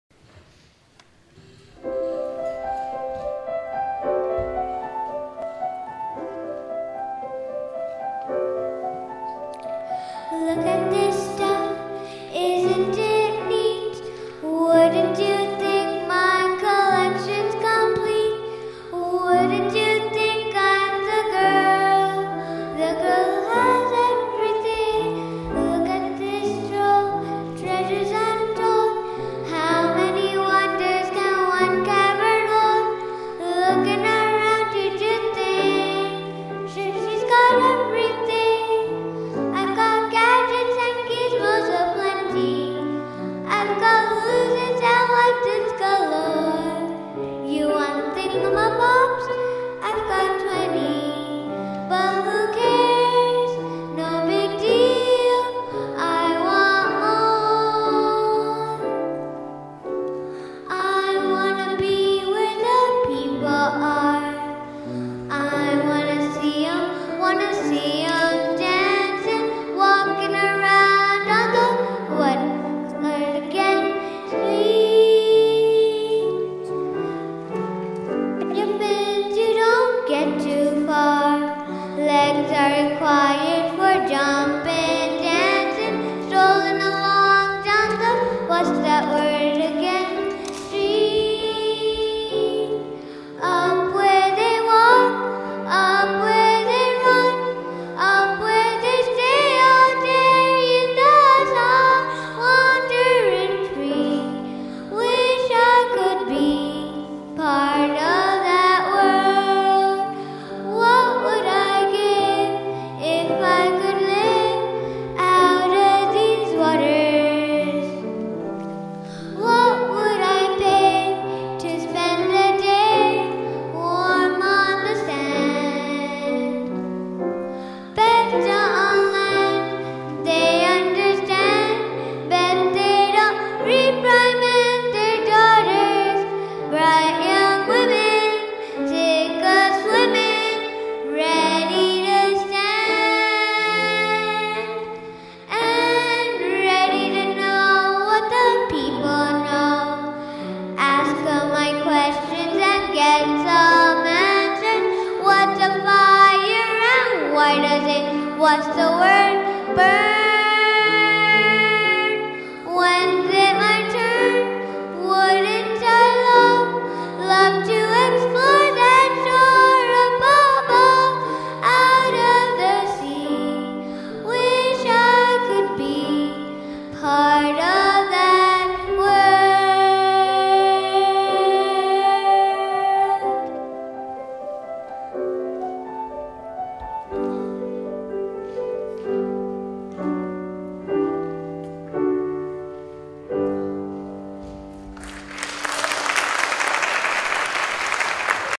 Nandana
Students from Veksler Academy of Music & Dance perform at Santa Clara university - December 2019